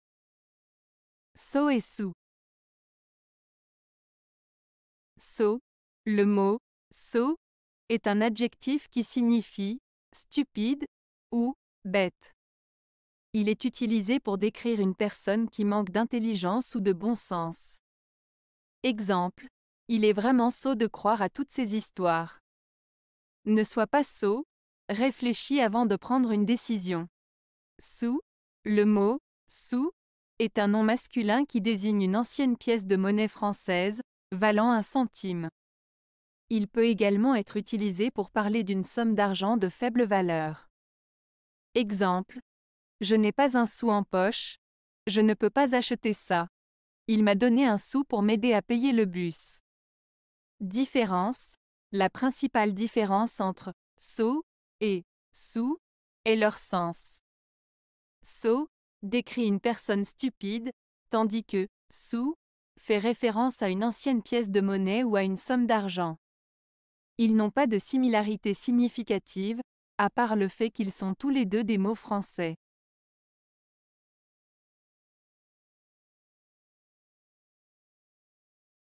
Sot et Sou (Une Lettre)
Sot:
Le mot "sot" est un adjectif qui signifie "stupide" ou "bête". Il est utilisé pour décrire une personne qui manque d'intelligence ou de bon sens.

Exemples:
- Il est vraiment sot de croire à toutes ces histoires.
- Ne sois pas sot, réfléchis avant de prendre une décision.

Sou:
Le mot "sou" est un nom masculin qui désigne une ancienne pièce de monnaie française, valant un centime. Il peut également être utilisé pour parler d'une somme d'argent de faible valeur.

Exemples:
- Je n'ai pas un sou en poche, je ne peux pas acheter ça.
- Il m'a donné un sou pour m'aider à payer le bus.

Différence:

La principale différence entre "sot" et "sou" est leur sens. "Sot" décrit une personne stupide, tandis que "sou" fait référence à une ancienne pièce de monnaie ou à une somme d'argent. Ils n'ont pas de similarités significatives, à part le fait qu'ils sont tous les deux des mots français.